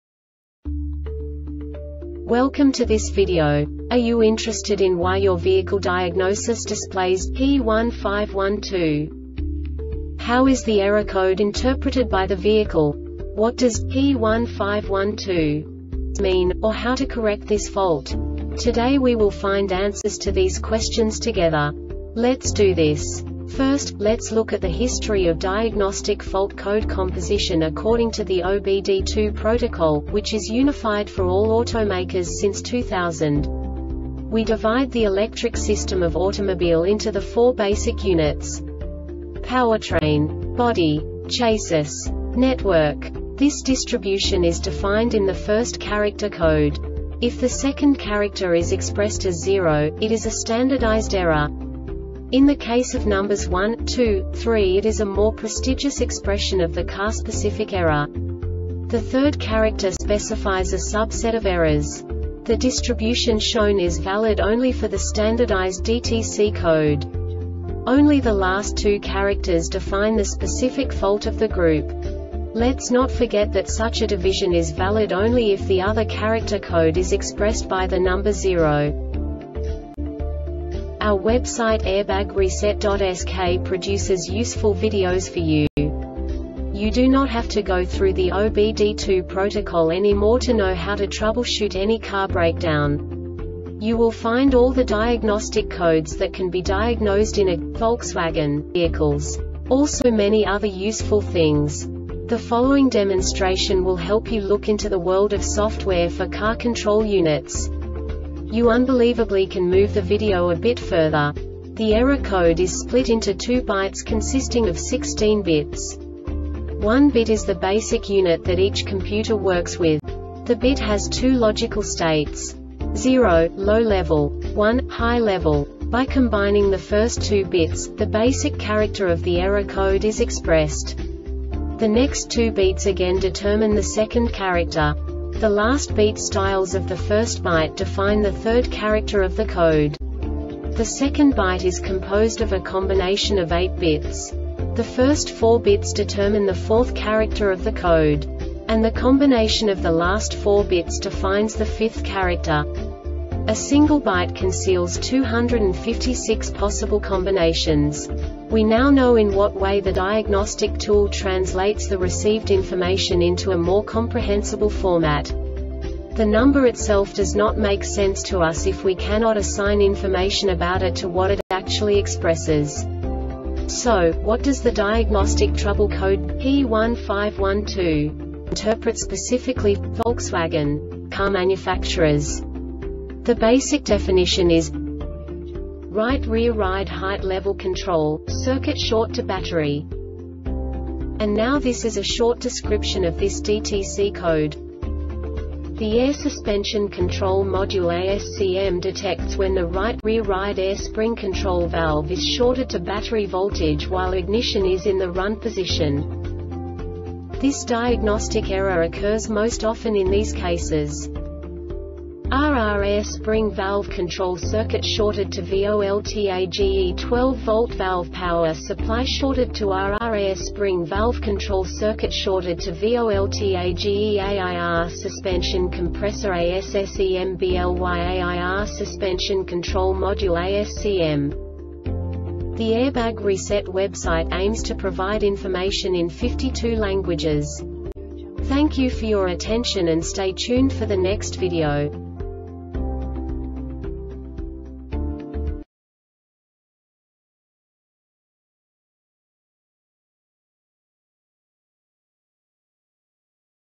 DTC Volkswagen P1512 Short Explanation
Contents:
0:21 Basic DTC analysis according to OBD2 protocol standard.
1:48 Insight into programming
2:58 A diagnostically understandable description of the Diagnostic Trouble Code
3:05 Basic error definition

Make:
Volkswagen
Code:
P1512
Definition:
Right Rear Ride Height Level Control - Circuit Short To Battery
Description:
The Air Suspension Control Module (ASCM) detects when the right rear ride air spring control valve is shorted to battery voltage while ignition is in the Run position.
Cause:
RR AIR SPRING VALVE CONTROL CIRCUIT SHORTED TO VOLTAGE12 VOLT VALVE POWER SUPPLY SHORTED TO RR AIR SPRING VALVE CONTROL CIRCUIT SHORTED TO VOLTAGEAIR SUSPENSION COMPRESSOR ASSEMBLYAIR SUSPENSION CONTROL MODULE (ASCM)